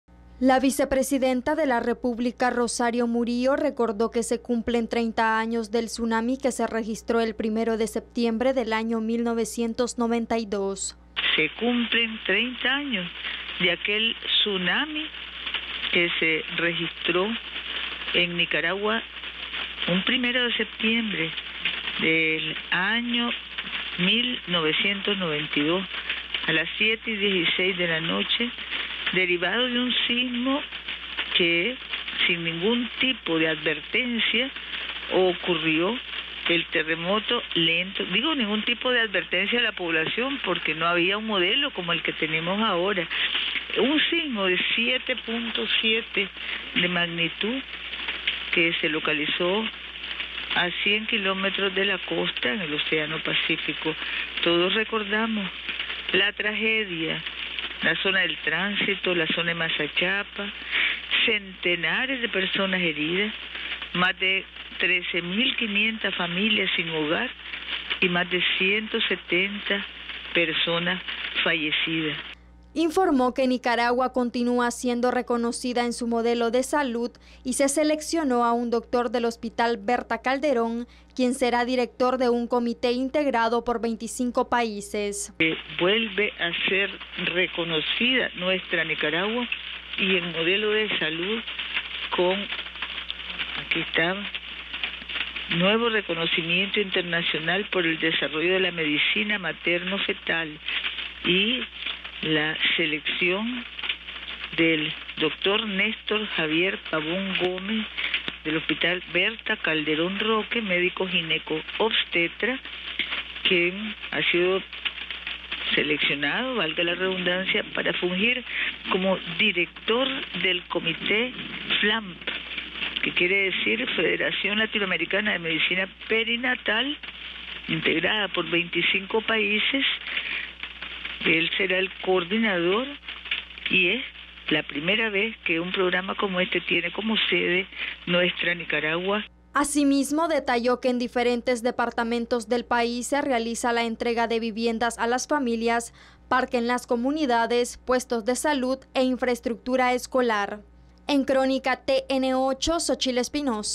Con honor Nicaragua destaca actividades en inicio al mes de la patria
Esta tarde la Vicepresidenta de Nicaragua, Rosario Murillo; informó al pueblo nicaragüense que en medio de las lluvias hay que poner en práctica todo lo que ayude a salvaguardar las vidas, y que corramos a atendernos como familia.